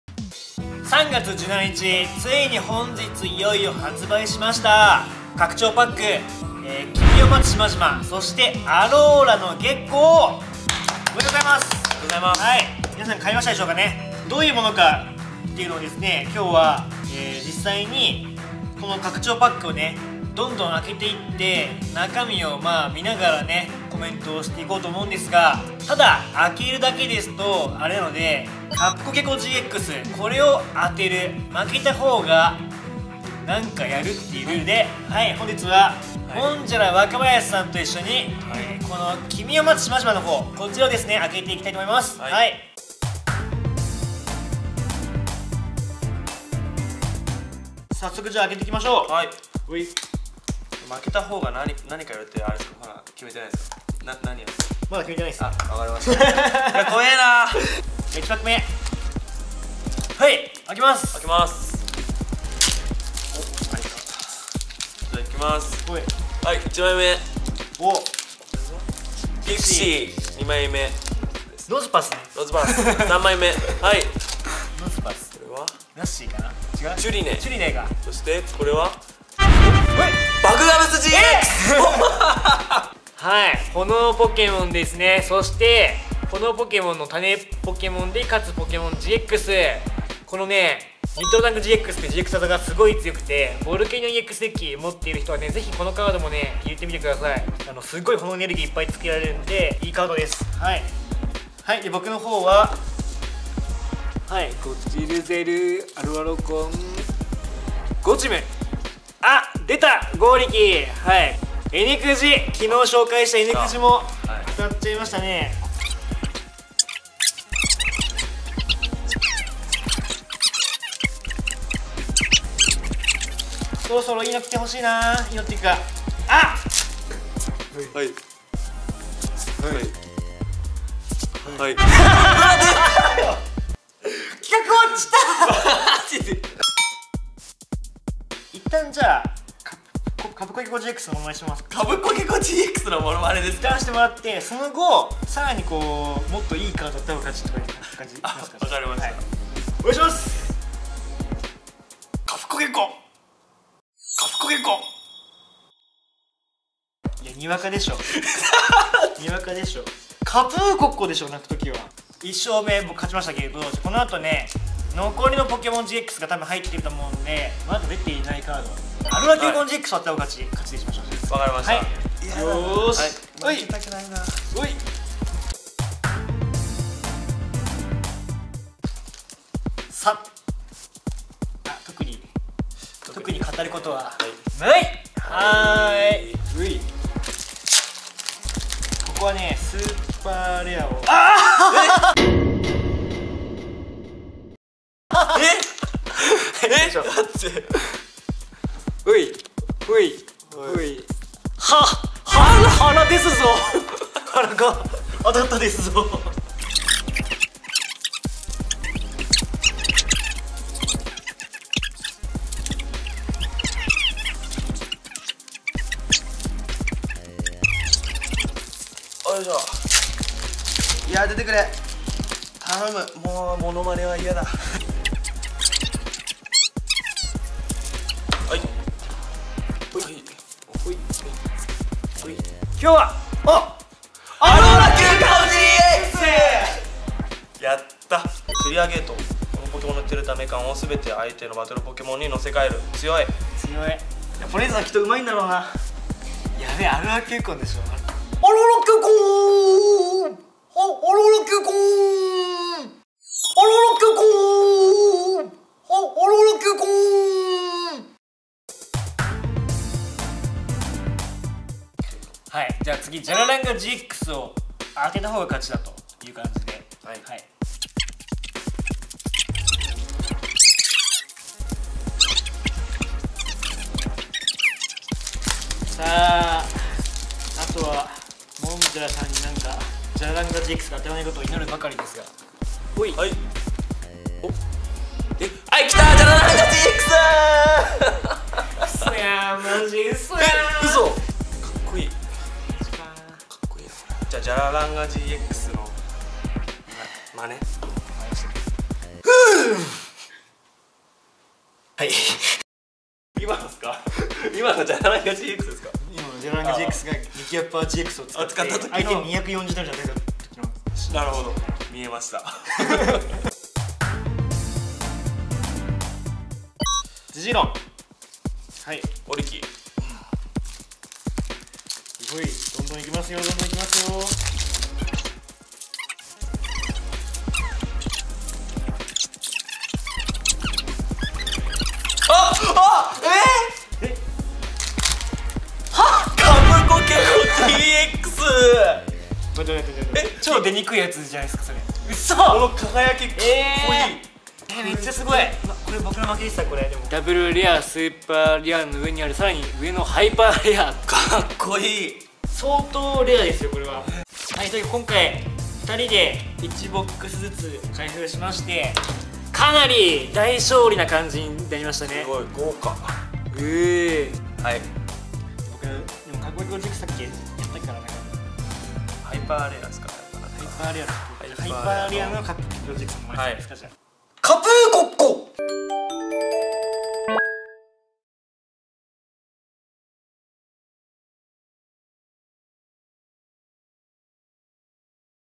カプ・コケコGXを当てろ！拡張パック「キミを待つ島々」開封対決
本日は拡張パック「キミを待つ島々」「アローラの月光」の発売日！
早速「キミを待つ島々」の拡張パックを開封してみたぞ！